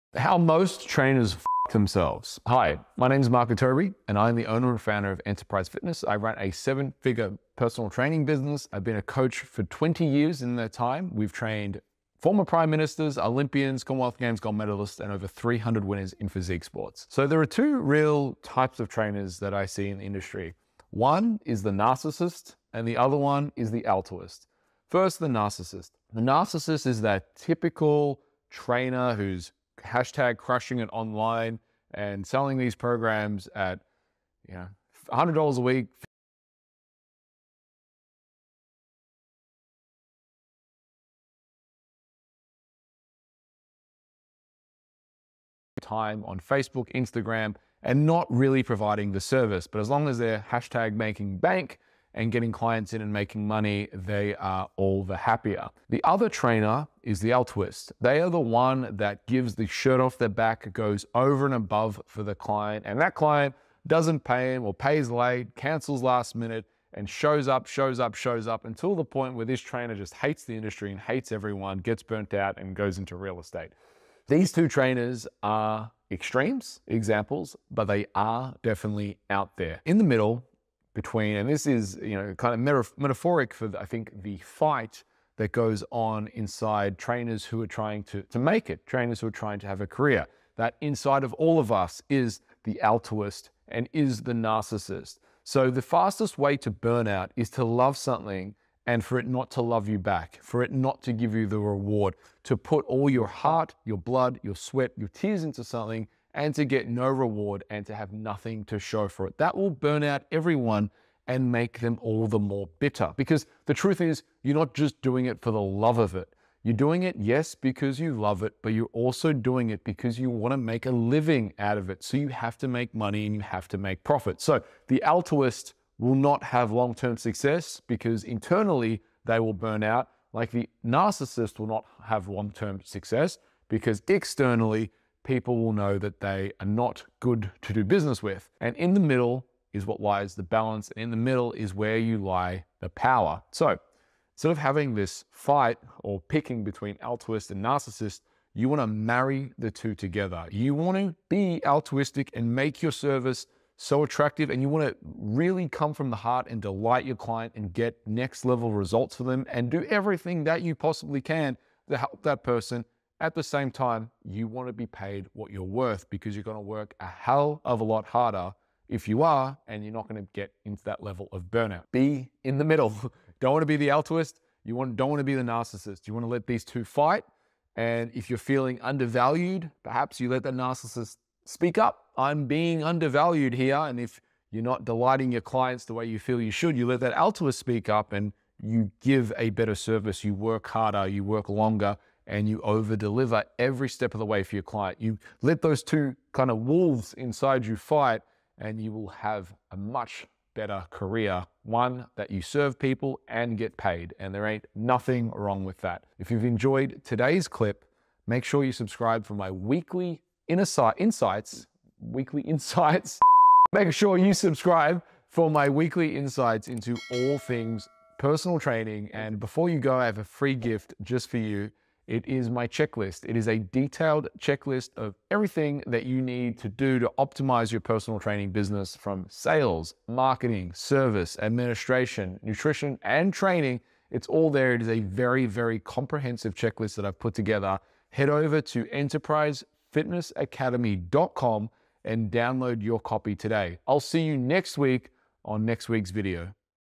Why Trainers Quit PT??
In this engaging and thought-provoking video, I delves deep into the common struggle personal trainers face: burnout and the potential path to quitting their career. Drawing inspiration from the ancient parable of the two wolves that reside within us - the narcissist and the altruist, I explore the inner battles that fitness professionals wage.

Join me as I uncover the signs of burnout, the emotional and physical toll it takes, and the reasons why many trainers hang up their coaching hats. But, this isn't just a tale of despair. Let's go further! Revealing strategies and insights for reigniting your passion, overcoming burnout, and finding harmony between the two wolves.

Whether you're a seasoned trainer or just starting your journey in the fitness industry, this video is a must-watch!